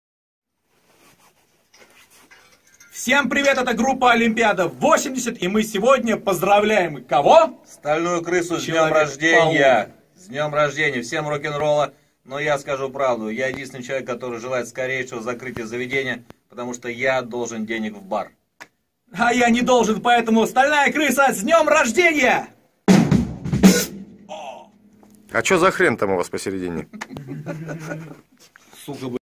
Поздравление от Олимпиады-80
Лидеры группы "Олимпиада-80", артисты, ведущие медийщики Волгограда Владимир Боднар и Михаил Григорян поздравляют бар "Стальная Крыса" с двухлетием.